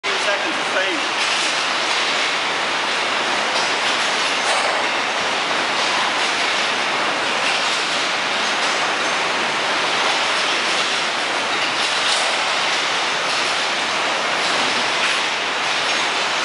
St. Thomas Assembly Plant - Facia Job
Anne hanging some fine multi colored facias - there's white and then there's white and.